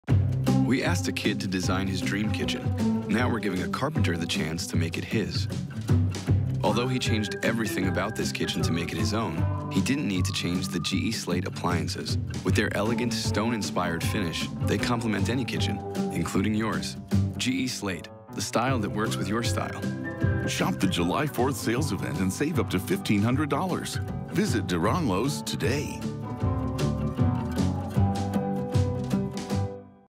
Deranleau's GE July 4th Sales Event
Save up to $1,500 on GE Appliances during the July 4th Sales event at Deranleau's!  Sale ends July 7th, 2016